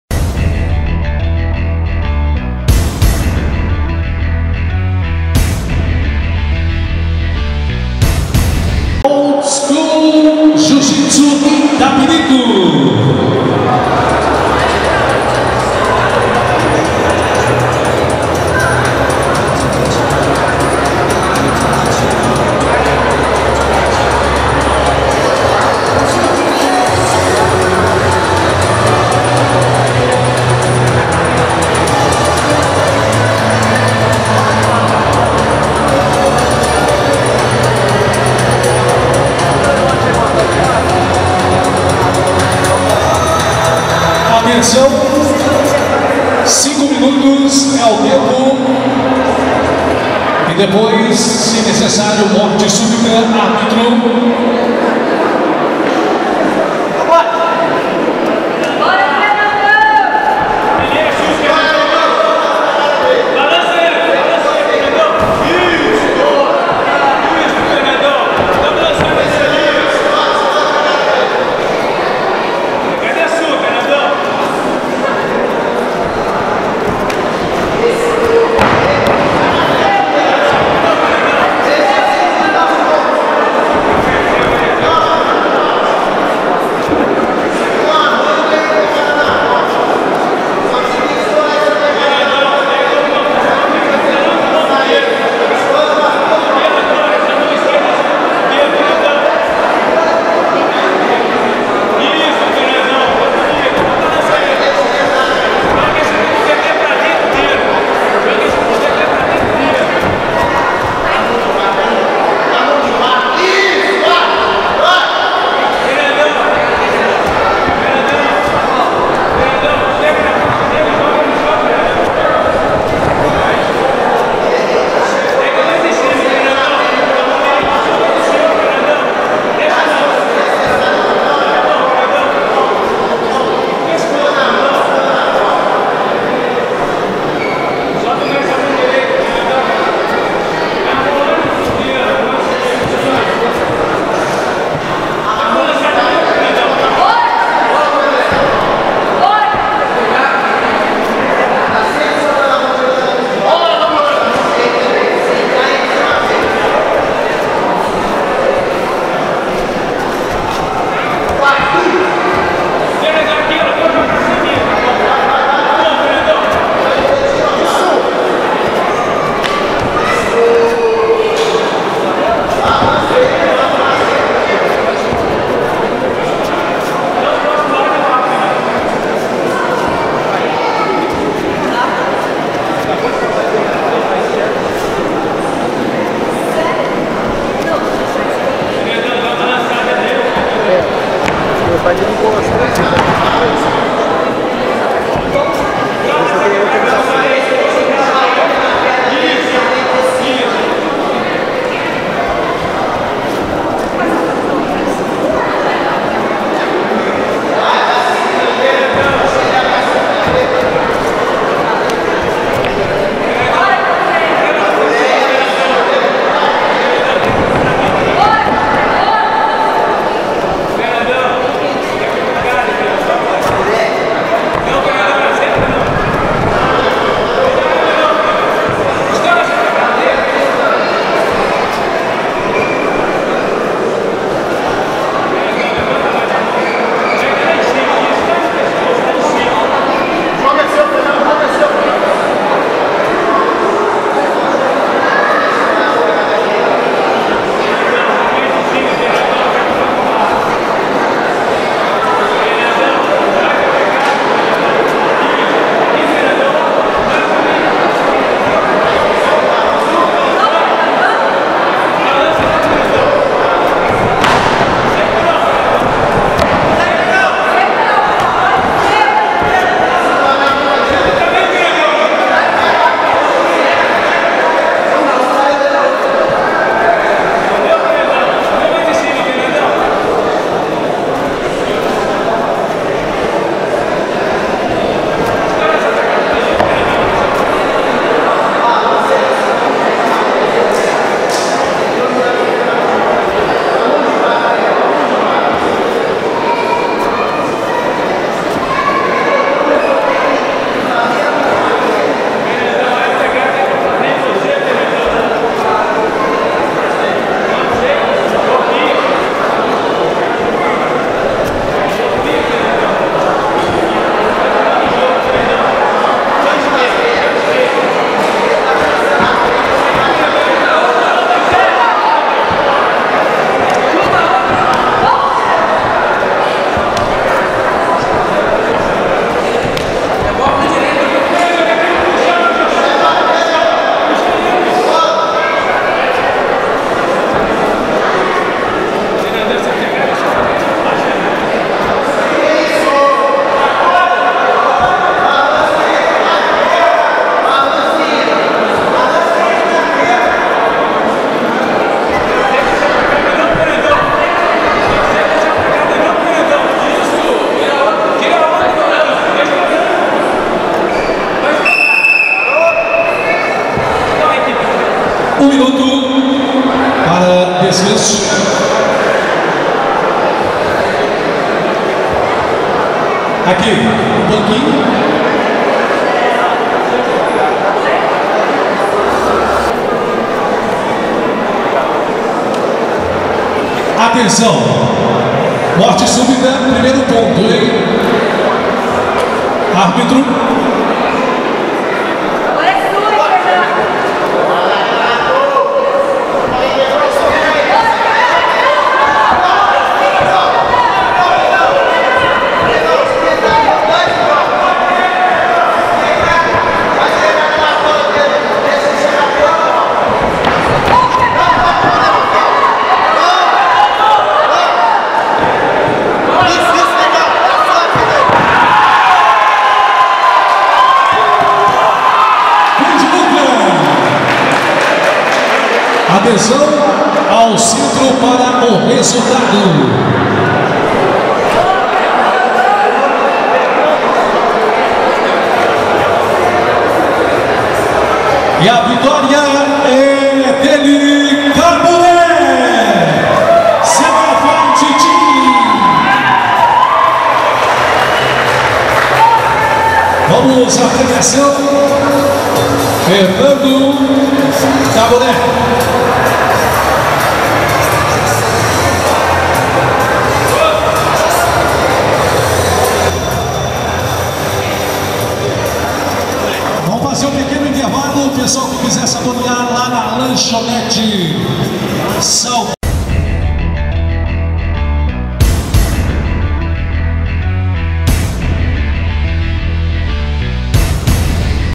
Caburé vs Renato - Open Fight Championship 2
Em combate de Jiu-Jitsu, Caburé (Sena Fight Team) venceu Renato (Old School Jiu-Jitsu).